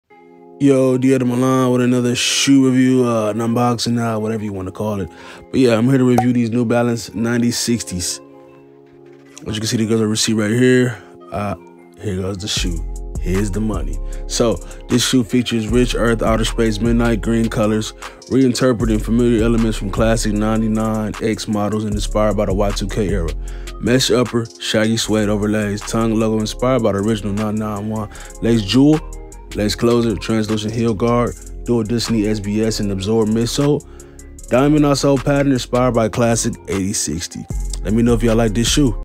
New Balance 9060 “Beef and Broccoli” 2023
Shoe Review + Unboxing New Balance 9060 "Beef and Broccoli" 2023 Release.